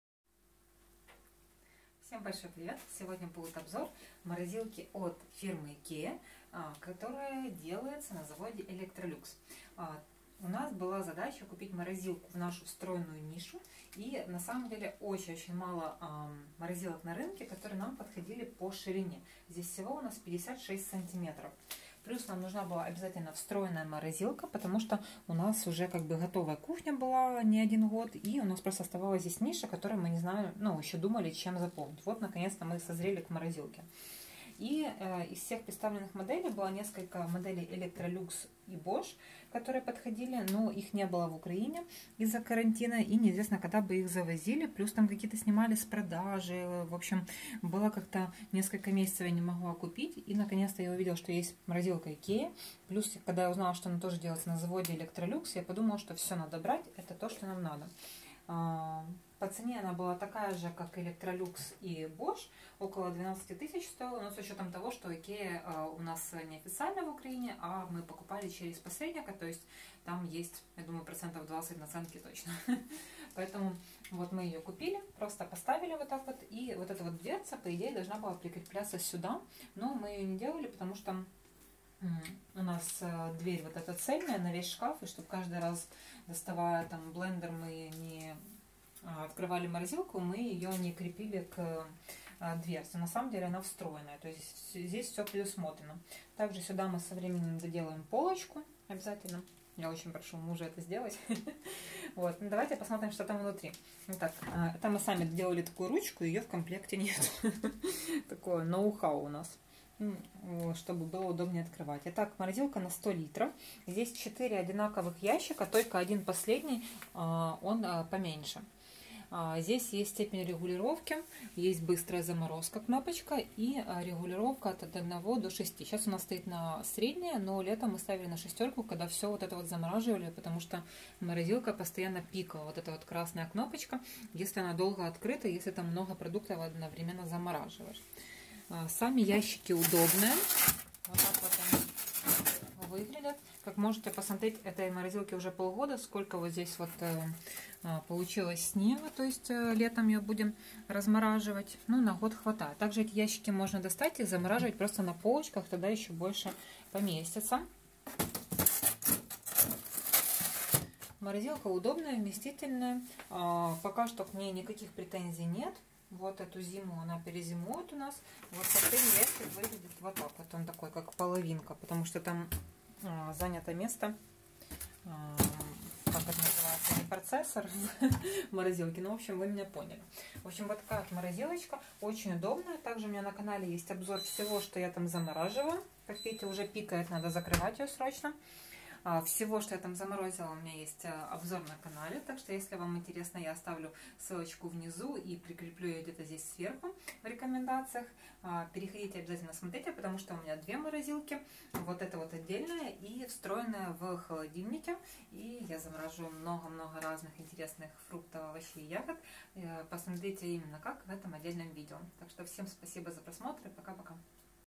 Обзор и отзыв про встраиваемый морозильник ikea DJUPFRYSA ДЬЮПФРЮСА икеа
Обзор и отзыв про встраиваемый
Компактный и энергоэффективный, с прозрачными ящиками, позволяющими видеть содержимое. Чтобы холодильник не выделялся на фоне интерьера, встройте его в шкаф и скройте за такой же дверцей, что и остальные фасады. Удачно сочетается с холодильником СВАЛЬНА.